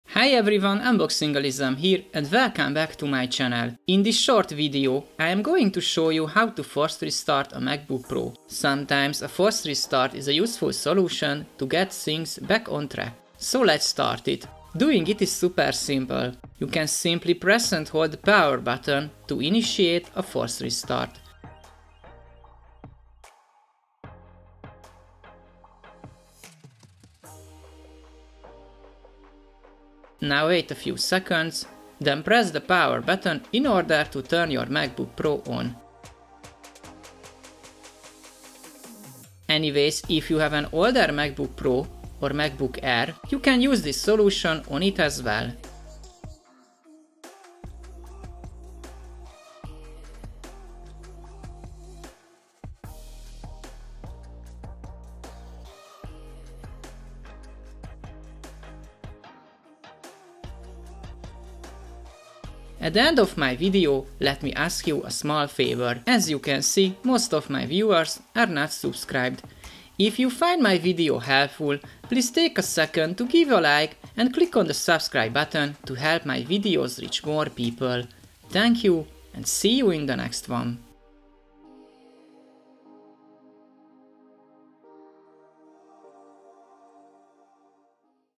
How To Force Restart Macbook Pro M1
The How To Force Restart Macbook Pro M1 video finally arrived to the channel!

00:00 - Intro
00:15 - How to force restart

I hope you will enjoy this how to force restart m1 macbook pro video, and I hope you will learn something new.